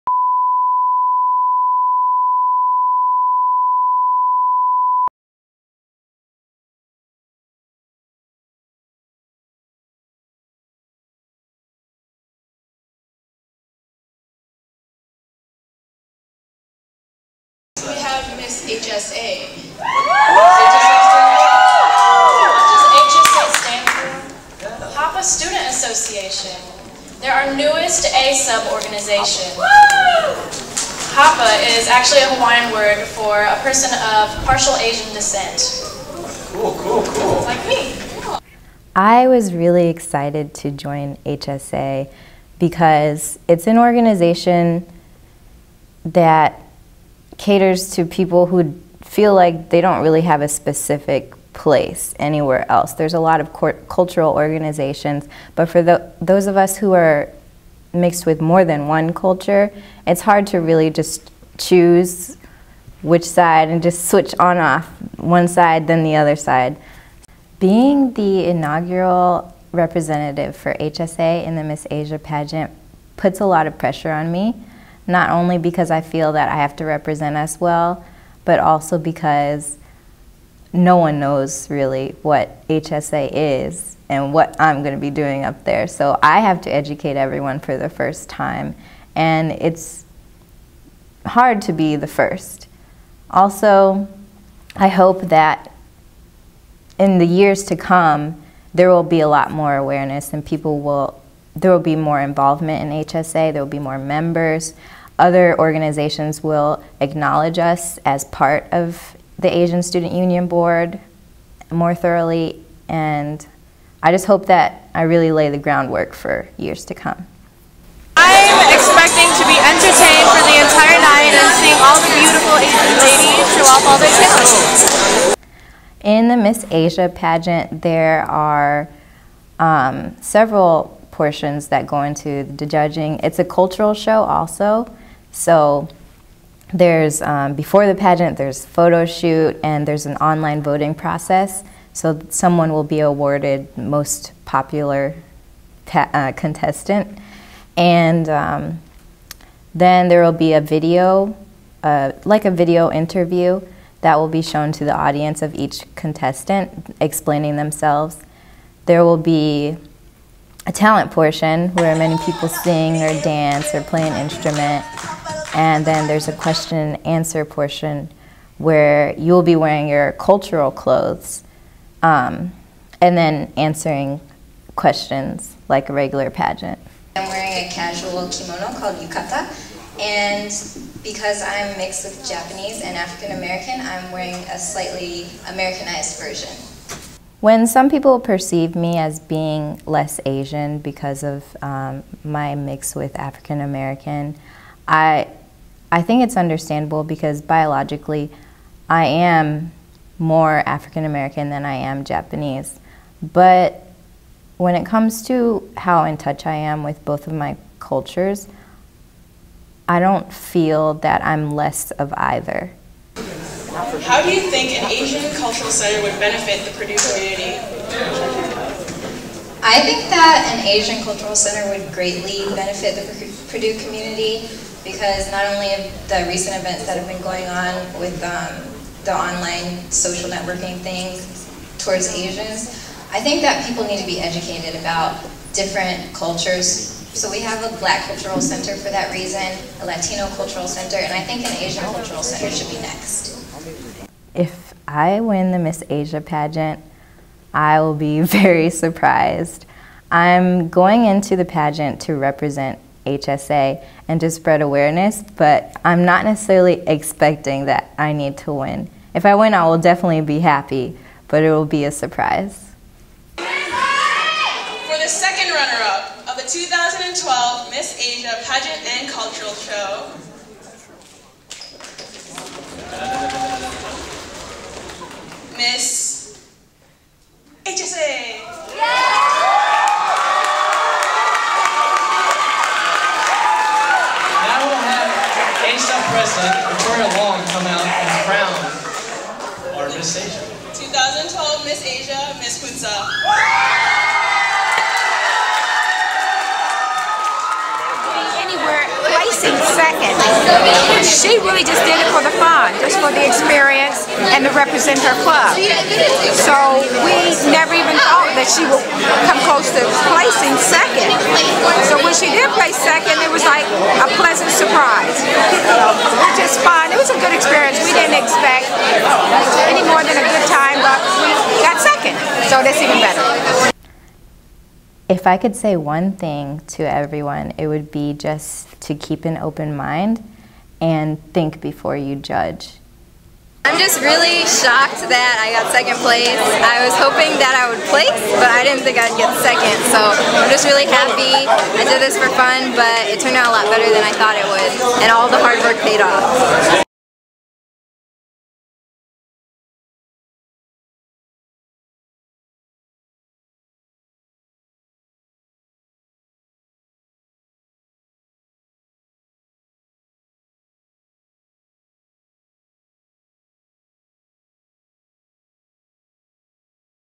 Transgressing Borders and Rethinking Identity (Biracial and Multicultural Identity)
Tiffany Carter is the first ever HSA (HAPA Student Association) representative for the Miss Asia Cultural Show at Purdue University in April 2012.